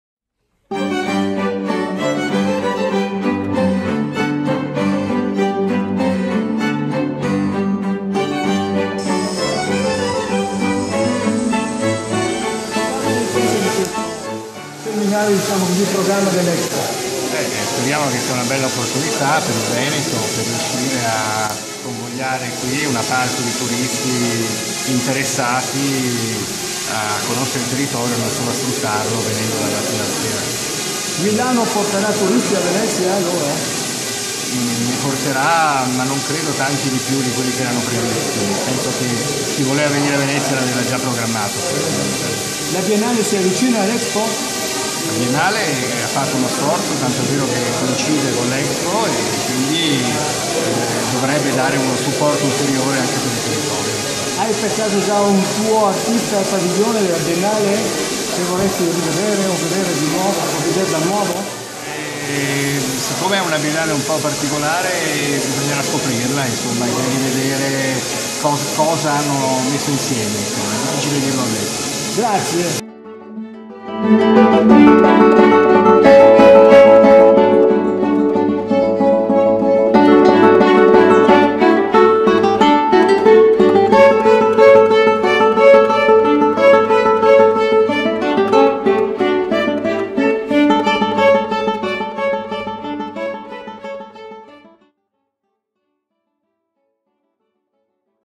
Intervista a Enrico Tantucci
Veneto per Expo 2015 - Regione Veneto -
Destinazione Veneto :
Il Programma di attività per le delegazioni estere -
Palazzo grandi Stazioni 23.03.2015